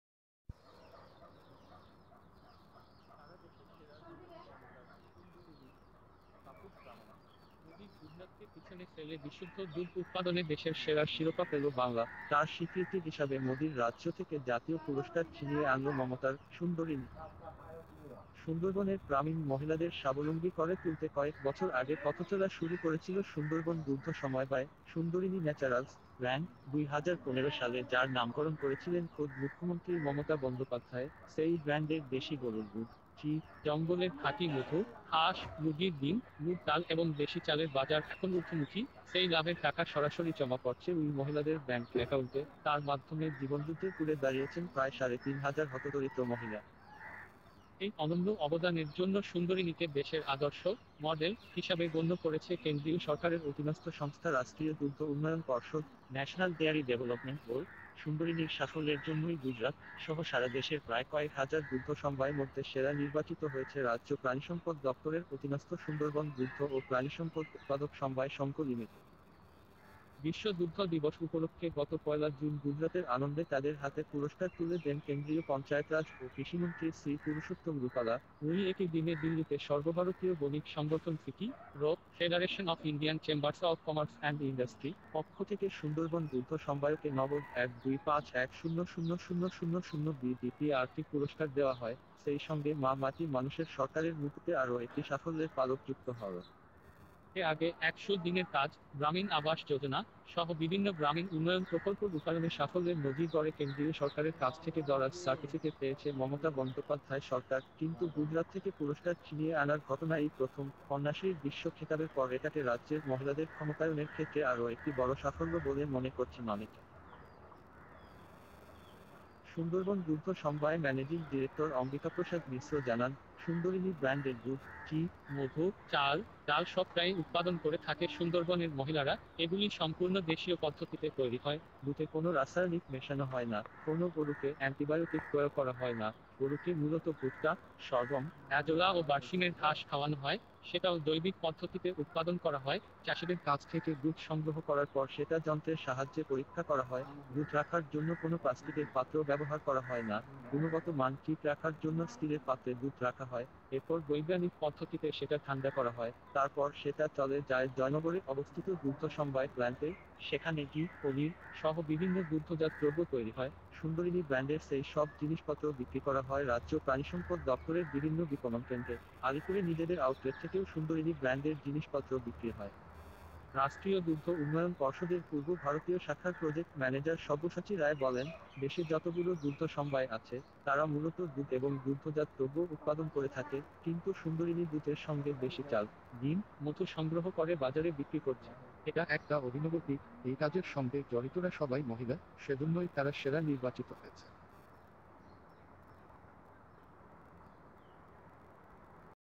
সারাদেশে সেরার মুকুট মমতার সুন্দরিনী'র।
সারাদেশে সেরার মুকুট মমতার সুন্দরিনী'র।এটা পশ্চিমবঙ্গ সরকারের সাফল্যের একটি দিক।